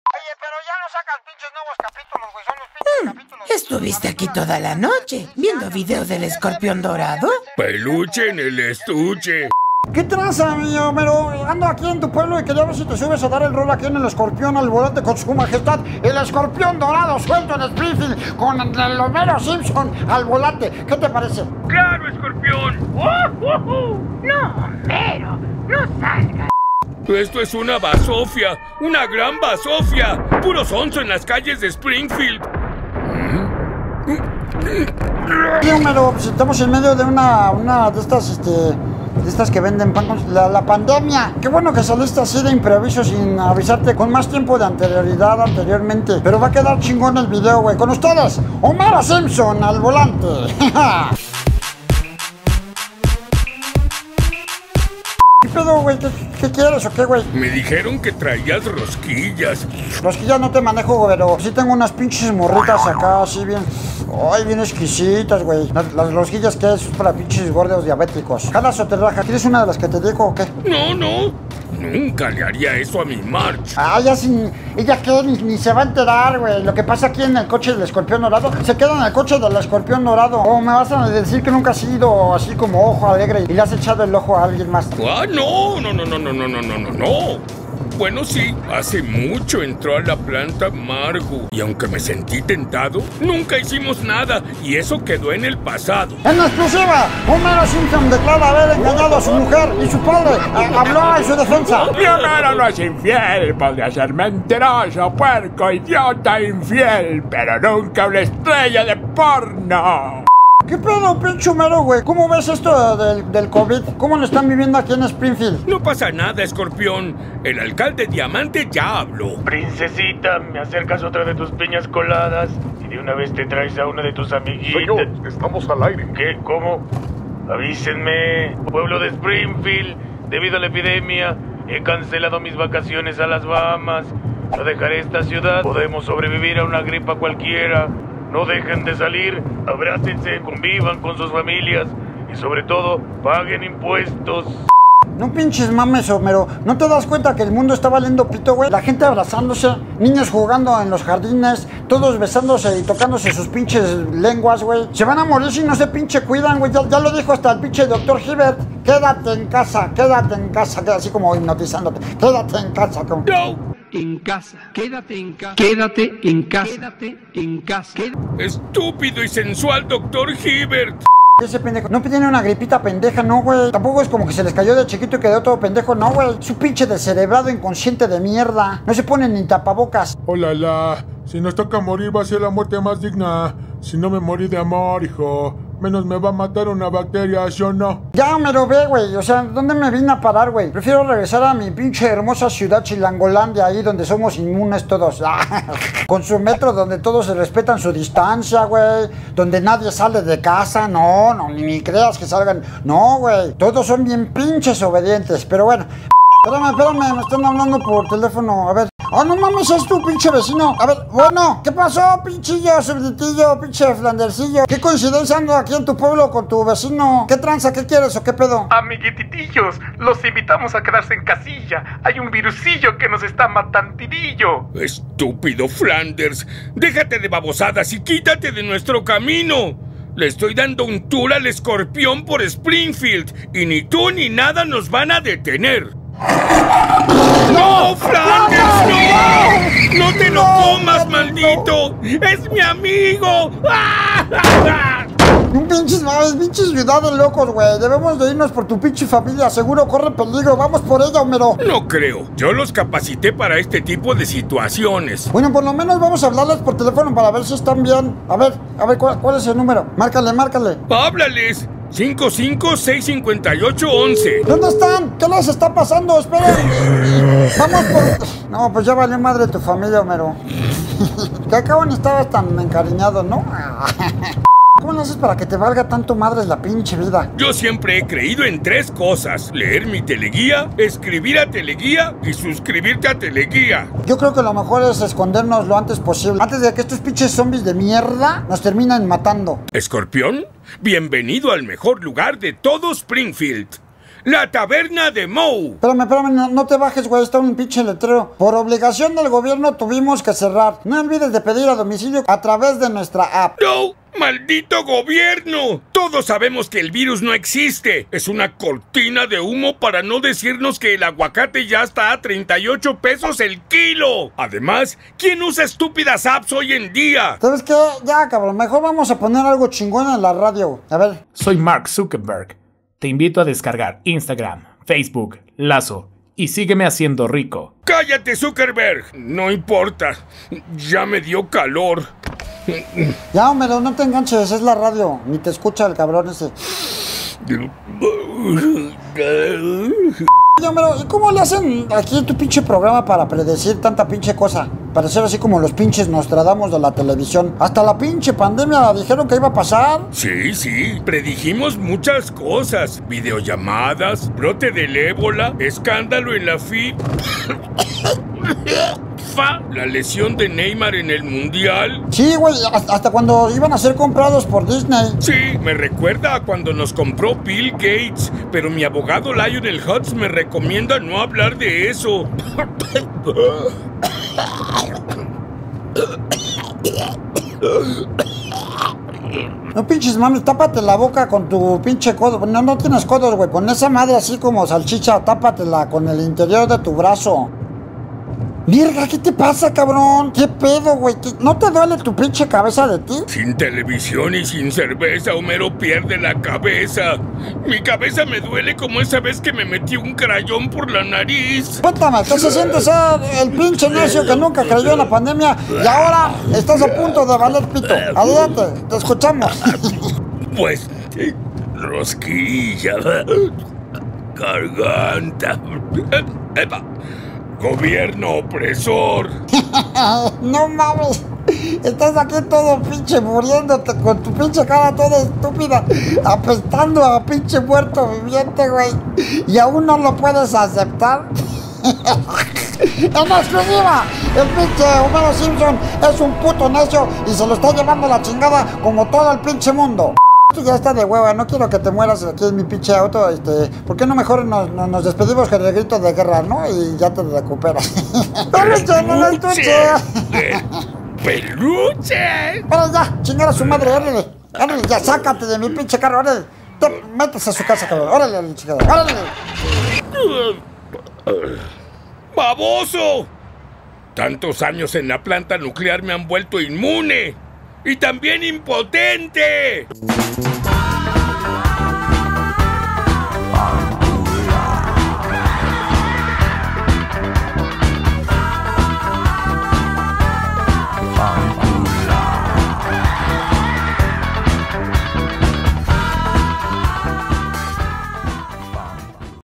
HOMERO & ESCORPIÓN AL VOLANTE, Springfield de cabeza!!!
Hoy su dios se fue hasta Springfield para dar un rol con un personaje que se dice ser mi fan.